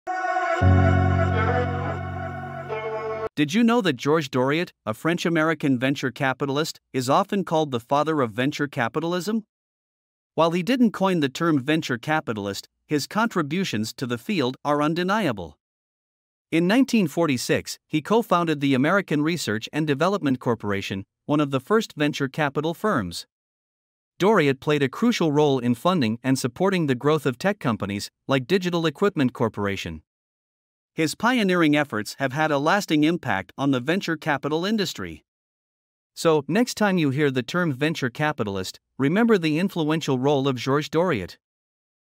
Georges Doriot: Revolutionizing VentureCapital | The Untold Story 🚀
Embark on a captivating journey through the life and legacy of Georges Doriot, the visionary force behind the evolution of VentureCapital. Uncover the untold story of the 'father of venture capitalism' and co-founder of American Research and Development Corporation (ARDC) in 1946. Explore the strategic brilliance behind Doriot's game-changing investments, including the iconic support for Digital Equipment Corporation (DEC). Join us in celebrating InnovationHistory and the enduring impact of a true VentureVisionary. Don't miss out – witness the evolution of venture capitalism with Georges Doriot, the trailblazer of TechEntrepreneurship! 🌐✨